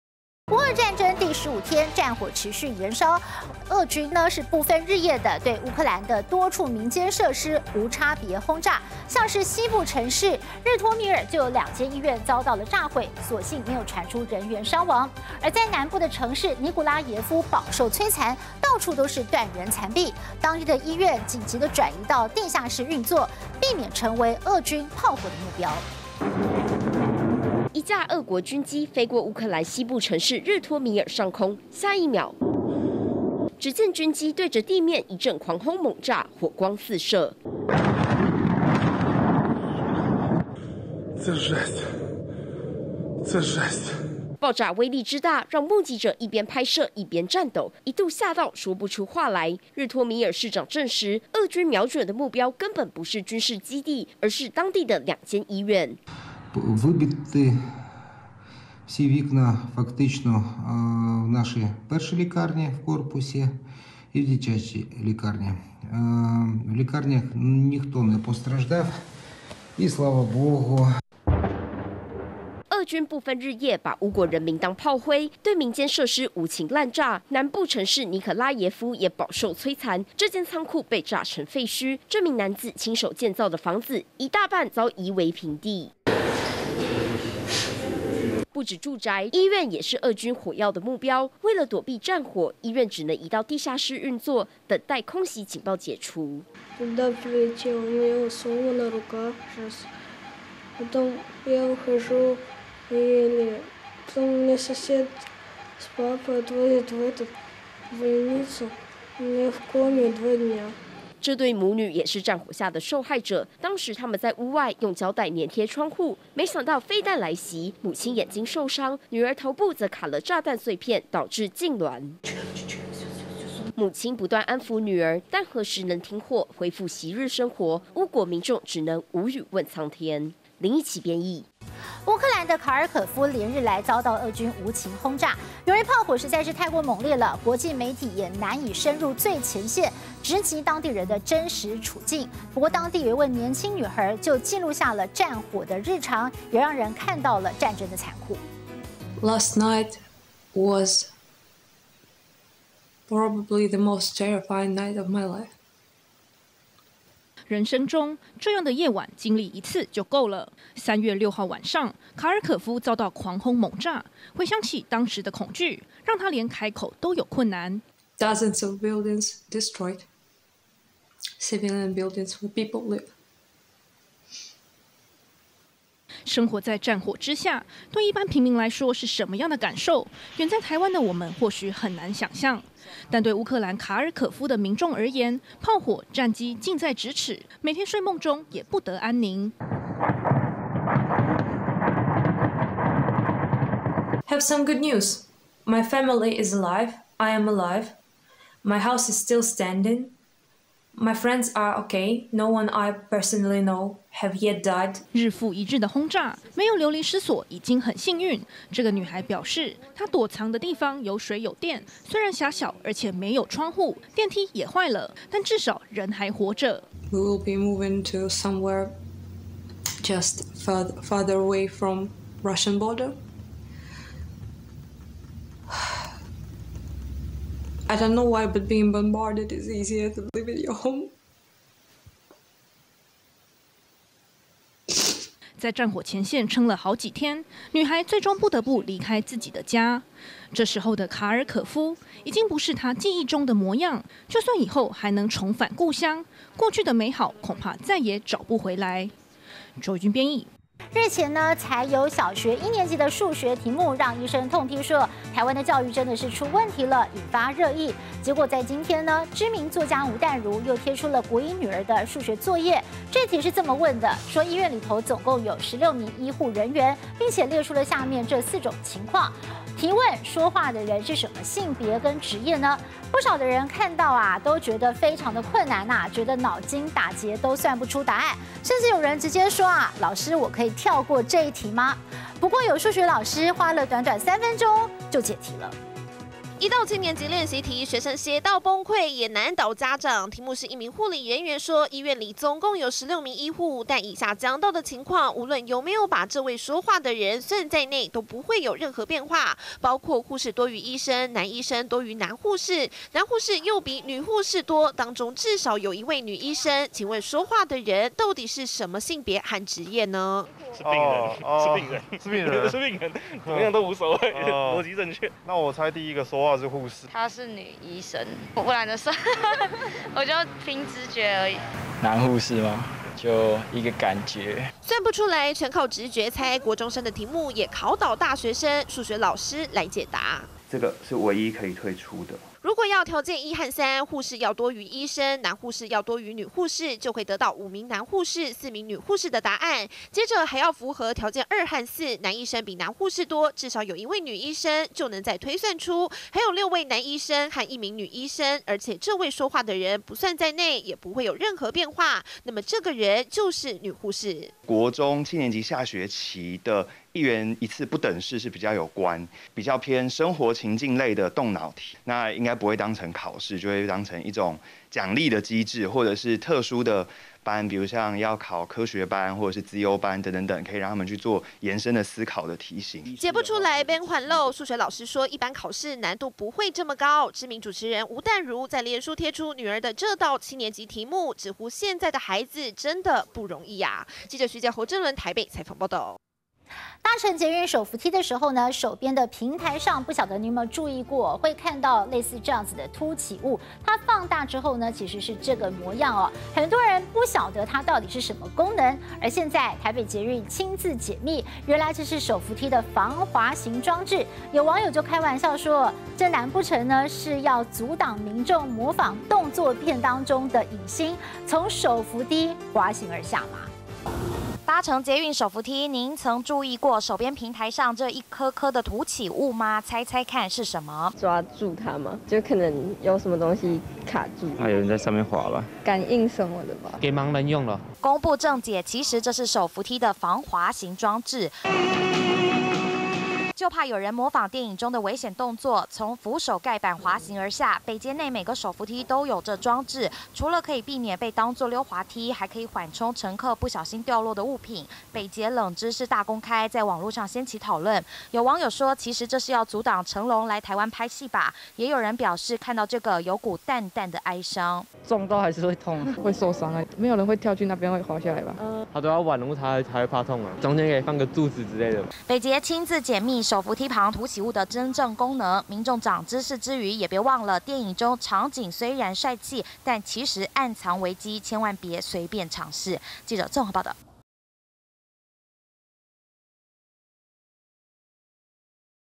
俄「無差別」轟炸 醫院民宅滿目瘡痍【發燒話題】-20220310
(00:00)俄軍「無差別」空襲 日托米爾2醫院遭炸毀
(02:11)卡爾可夫遭狂轟猛炸！女孩紀錄戰火下日常
(04:10)醫護16人考「我是誰」 7年級題目難倒千人
(06:26)捷運電扶梯整排突起物 網笑：防「成龍」裝置